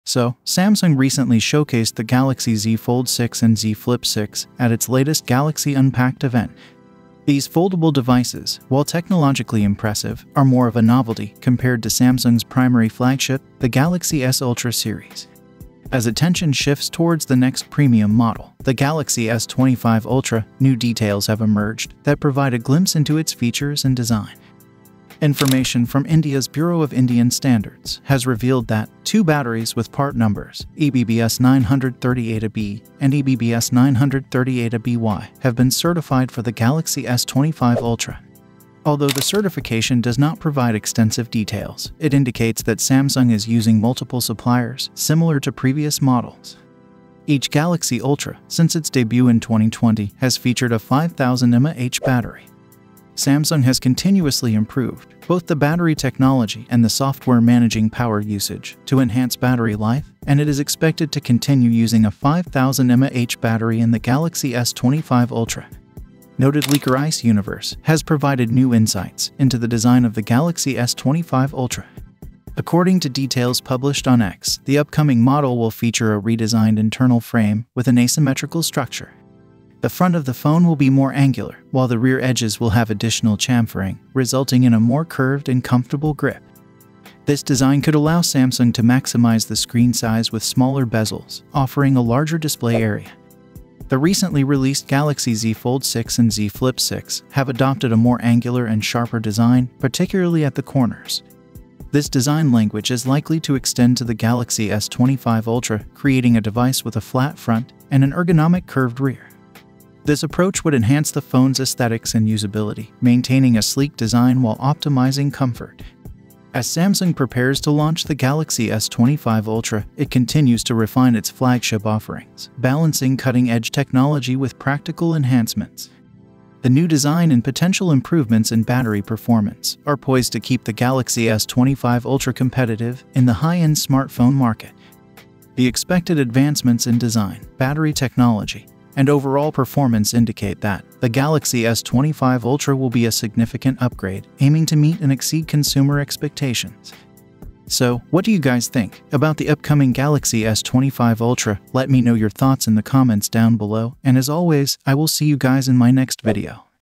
Samsung Galaxy S25 Ultra - HERE IT IS!
Samsung recently showcased the Galaxy Z Fold6 and Z Flip6 at its latest Galaxy Unpacked event. These foldable devices, while technologically impressive, are more of a novelty compared to Samsung's primary flagship, the Galaxy S Ultra series. As attention shifts towards the next premium model, the Galaxy S25 Ultra, new details have emerged that provide a glimpse into its features and design.